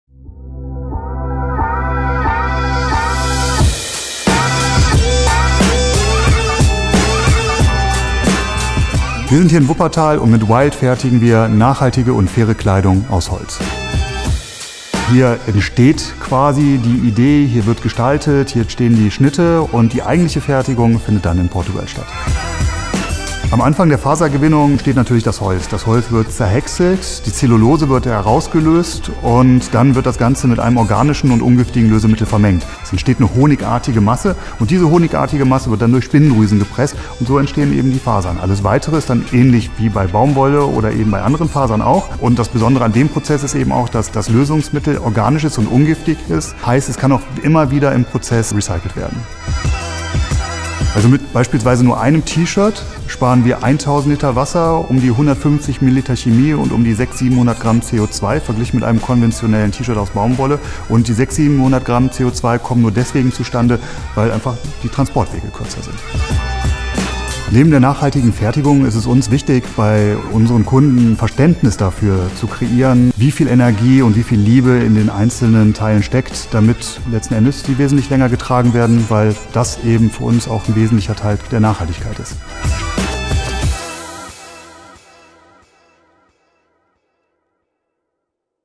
Das Wuppertaler Textillabel wijld gewinnt Effizienz-Preis NRW 2019 für Kleidung aus Holzfasern
Das Wuppertaler Textillabel wijld erhielt am 19. November 2019 den Effizienz-Preis NRW für die Herstellung von wertigen Basic-Kleidungsstücken aus nachhaltigen Holzfasern mit dem Ziel, schnelllebige Wegwerfmode durch nachhaltige und langlebige Produkte zu ersetzen.

Der „Effizienz-Preis NRW – Das ressourceneffiziente Produkt“ wird von der Effizienz-Agentur NRW verliehen, die im Auftrag des nordrhein-westfälischen Umweltministeriums arbeitet. Der Preis würdigt innovative Produkte und Dienstleistungen von kleinen und mittleren Unternehmen aus Nordrhein-Westfalen, die wesentliche Effizienzfaktoren miteinander verbinden – beispielsweise eine ressourcenschonende Produktentwicklung, umweltgerechte Herstellung und reduzierte Umwelteinflüsse während des Produktlebens sowie eine umfassende Recyclingfähigkeit. Der Preis ist mit insgesamt 20.000 Euro dotiert.

Das Textillabel wijld ist einer von vier Hauptpreisträgern des Effizienz-Preises NRW. Die Auszeichnung wurde am 19. November 2019 im Deutschen Sport & Olympia Museum in Köln durch die nordrhein-westfälische Umweltministerin Ursula Heinen-Esser überreicht. Neben den vier Hauptpreisen vergab die Expertenjury auch einen Sonderpreis.